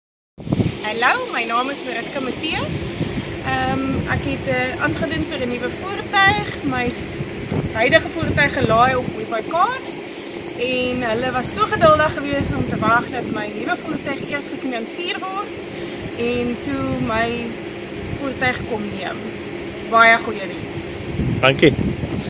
Happy Clients | WeBuyCars
Check out this week's TestimonialOfTheWeek. She was delighted with the service she received when trading in her vehicle at WeBuyCars. She noted how patient and accommodating the staff was with the process.

Visit webuycars.co.za to trade in your vehicle today!